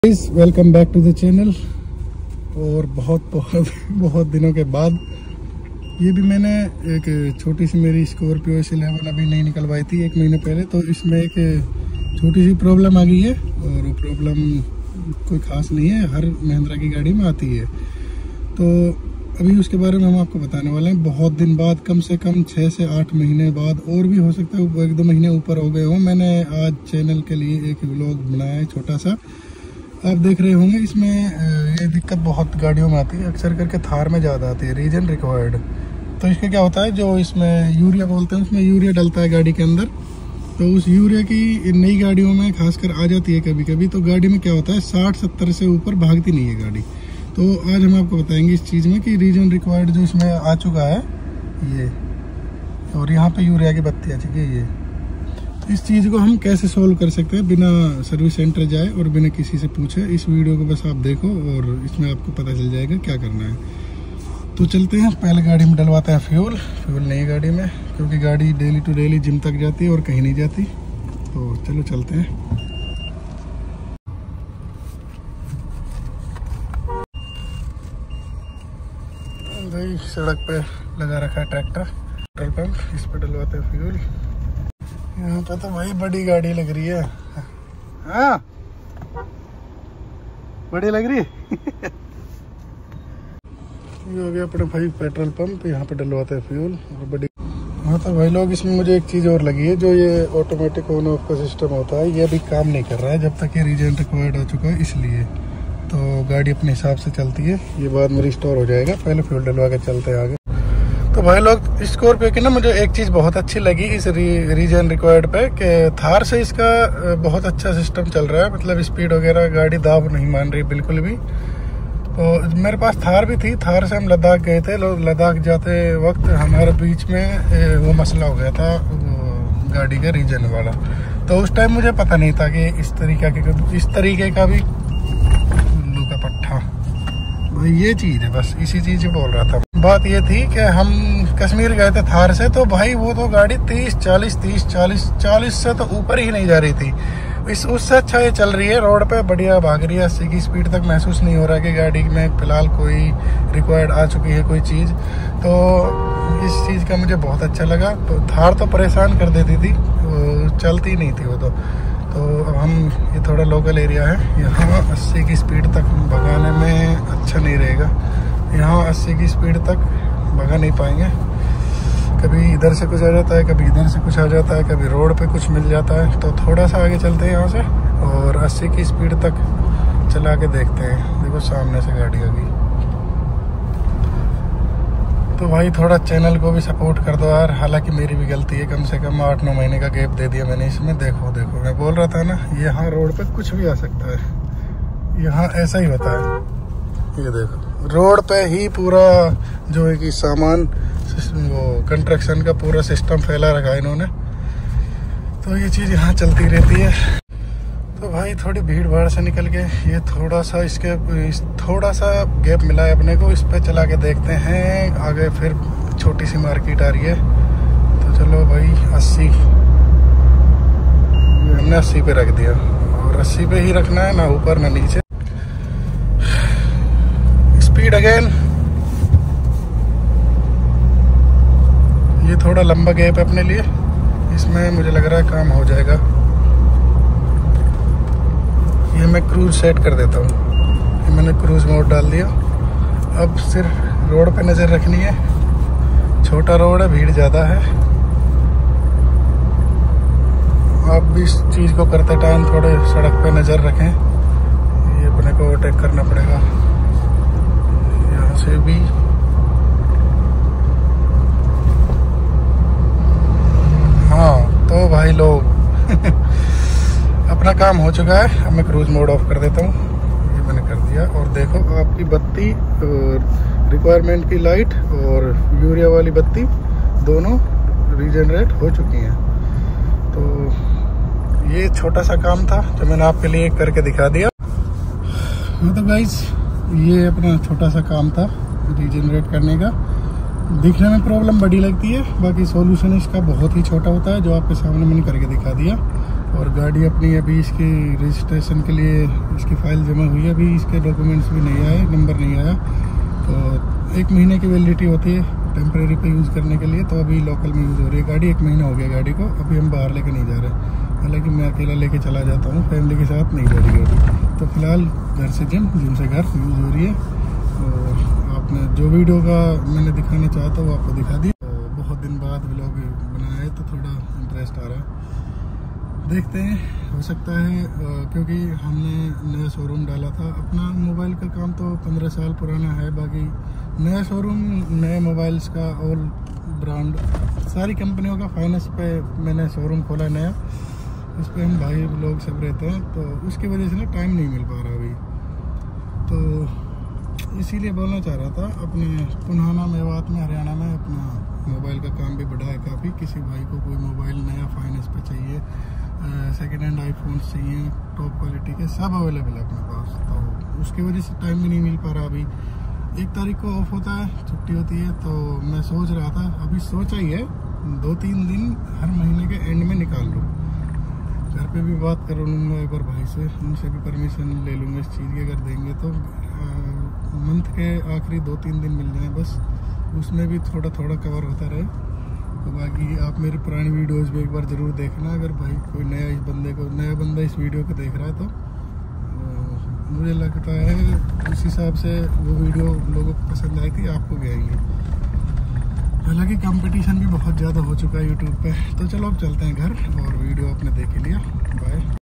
Mahindra Scorpio S11 classic Regen problem ⚠️ 5 मिनट मैं solution 👍✨ @AltafSampatvlogs thar
Mahindra Thar or scorpio Regen required solution here